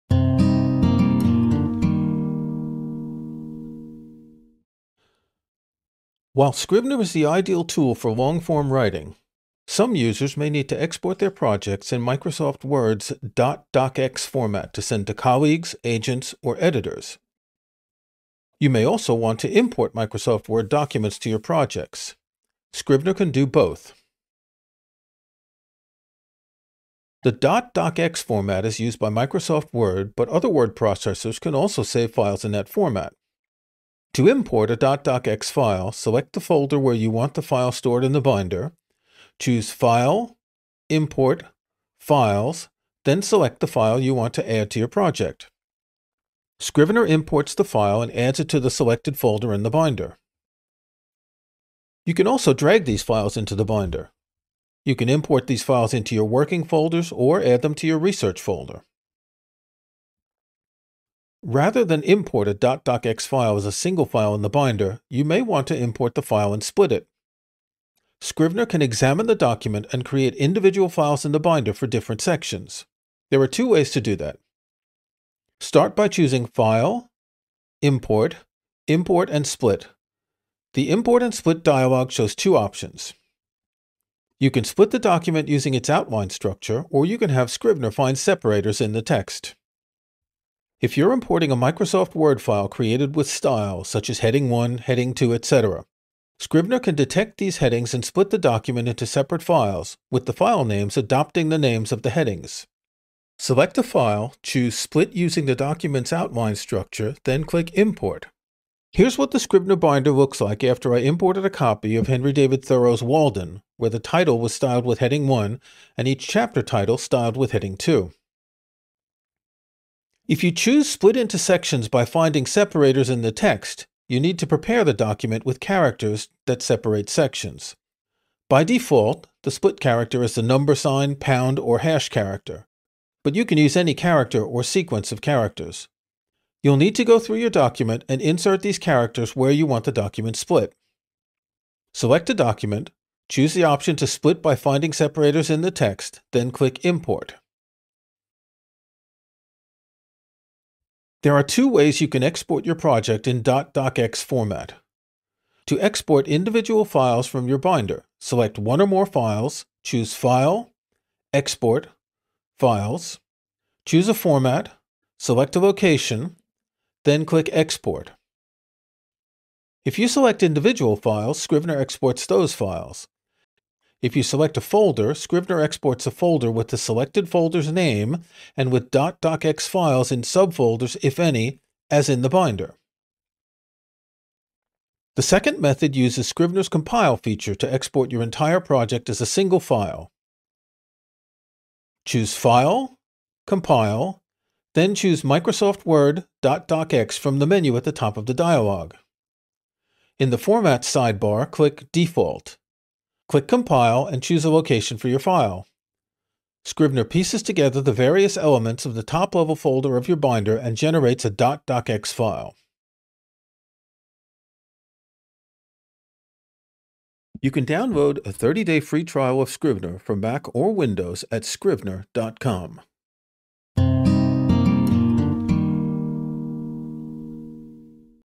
Scrivener and Word | Import, Split & Export Files
Whether your draft starts in Word, or needs to end there, Scrivener gives you the flexibility to work across tools without losing structure.

 What you’ll discover:

 ✅ Import full Word files into Scrivener
 ✅ Automatically split by headings or section markers
 ✅ Export scenes, chapters, or full drafts to clean .docx files
 ✅ Use Compile to generate editor-ready output